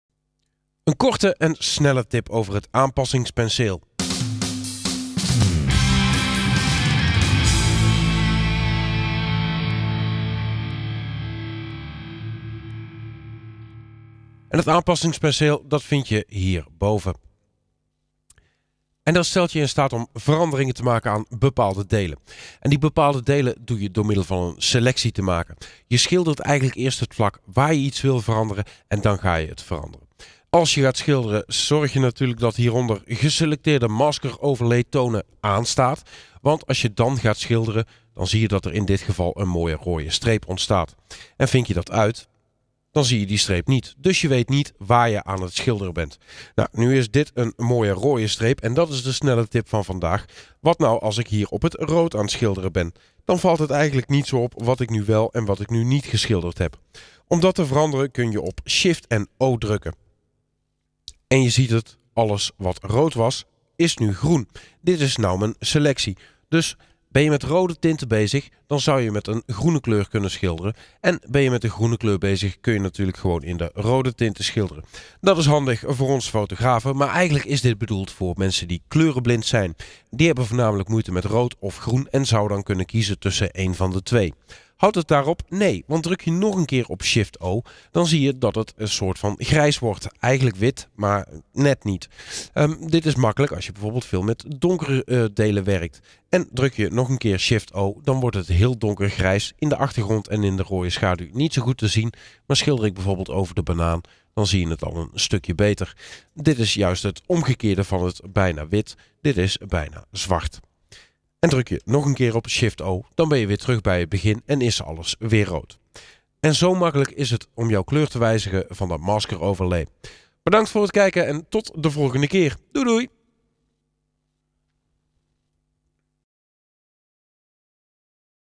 De kleur van je selectiemasker veranderen (Lightroom Classic)
Wanneer je met het aanpassingspaneel een selectie maakt, dan kun je die selectie zien. Dit heet de maskeroverlay. Deze is standaard rood gekleurd. Maar het is niet moeilijk om deze ook in het groen te tonen, of in het grijs of bijna zwart.

Speciaal voor visueel gehandicapten heeft Adobe deze kleurtjes bedacht. Voor iemand die kleurenblind is, is het soms makkelijker om met rood of groen te werken of juist met grijs of bijna zwart. En voor fotografen die geen problemen met kleuren hebben is het natuurlijk soms ook handig.